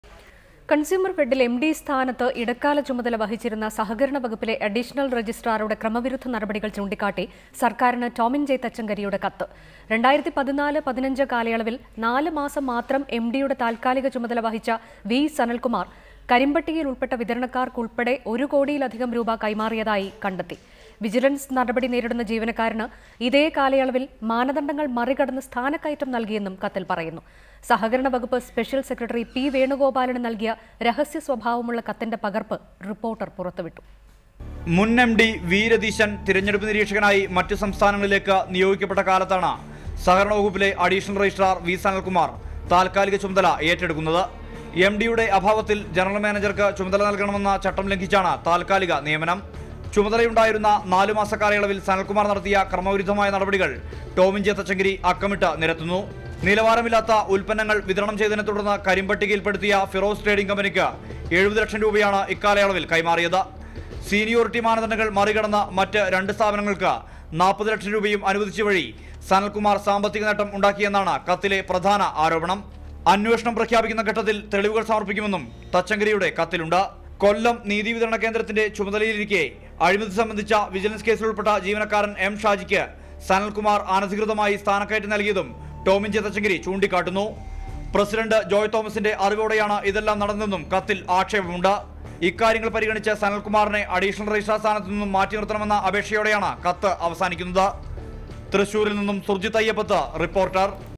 Consumerfed corruption: Tomin J Thachankary sent letter to Government
കണ്‍സ്യൂമര്‍ഫെഡില്‍ എംഡി സ്ഥാനത്ത് ഇടക്കാല ചുമതല വഹിച്ചിരുന്ന സഹകരണവകുപ്പിലെ അഡീഷണല്‍ രജിസ്ട്രാറുടെ ക്രമവിരുദ്ധ നടപടികള്‍ ചൂണ്ടിക്കാട്ടി സര്‍ക്കാരിന് ടോമിന്‍ ജെ തച്ചങ്കരിയുടെ കത്ത്.